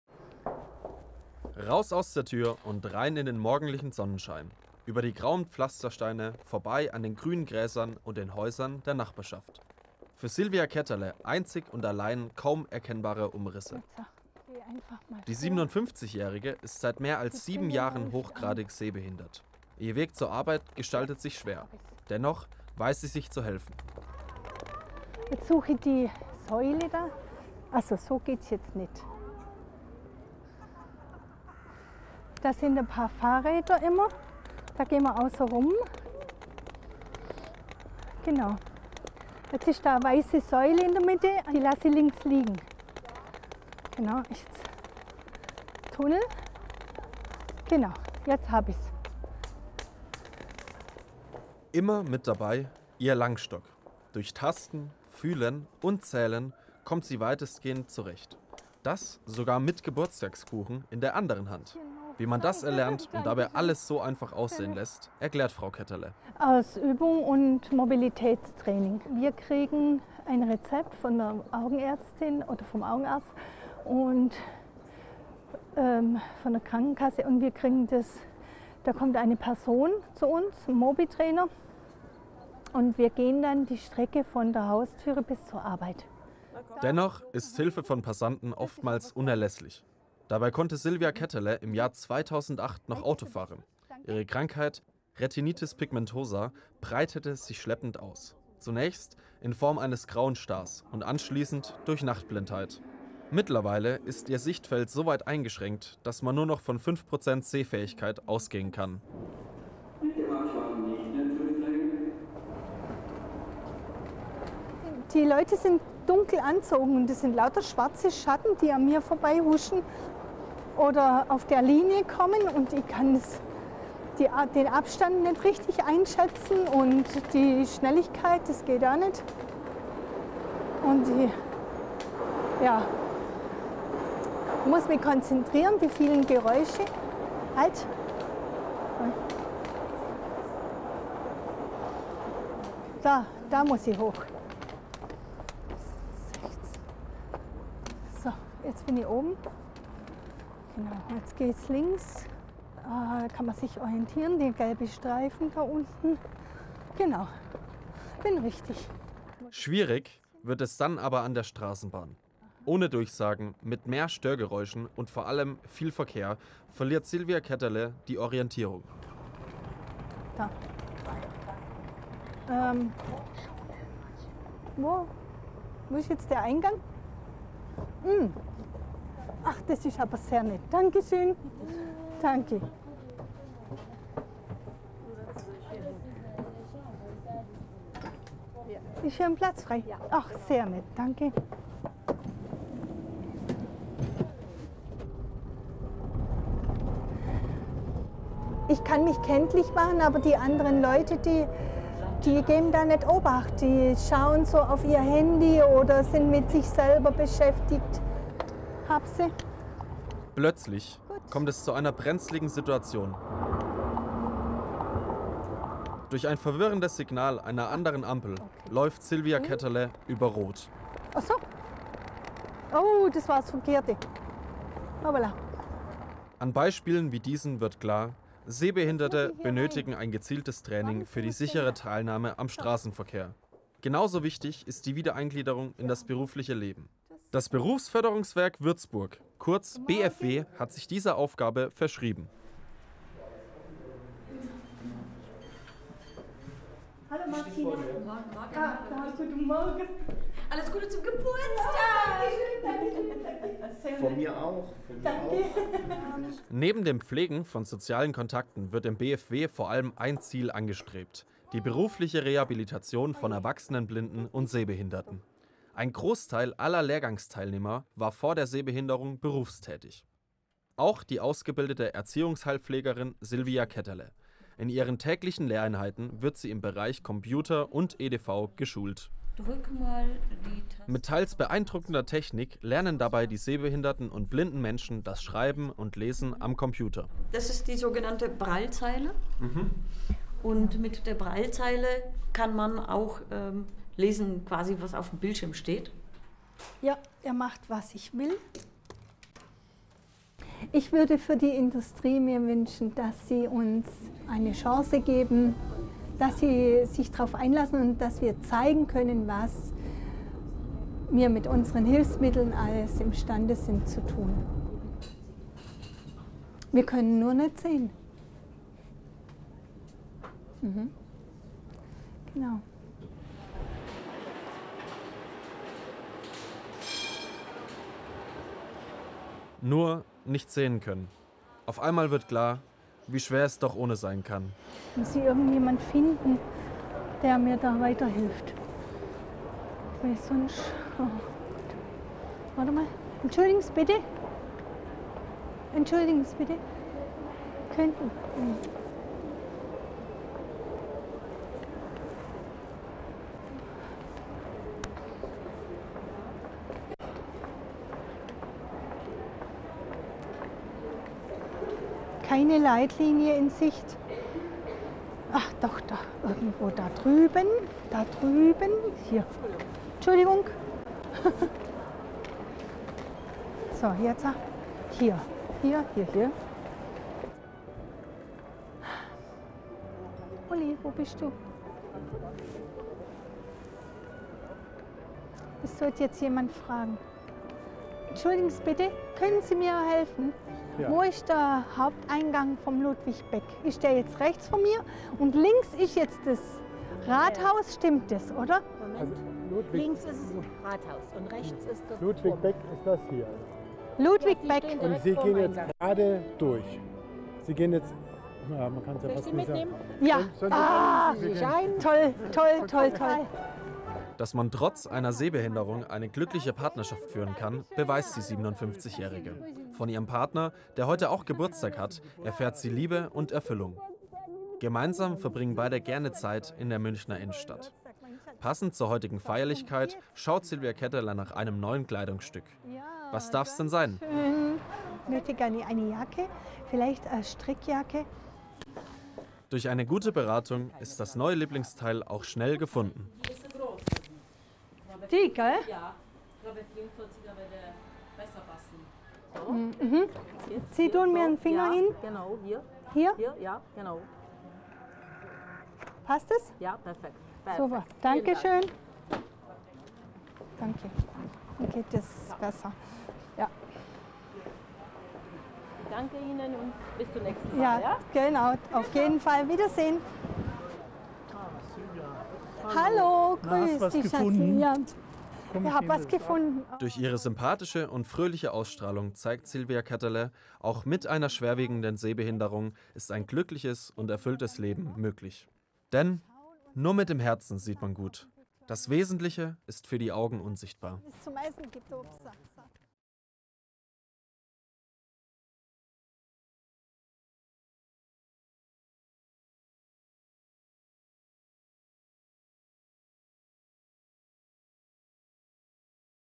Alltag mit Sehbehinderung
Wie ist es, als sehbehinderter Mensch in einer Großstadt zu leben? Mit all dem Stress, dem Lärm und auch Hindernissen muss Silvia Ketterle tagtäglich kämpfen – und führt dennoch ein glückliches Leben. Wir haben sie einen Tag lang begleitet.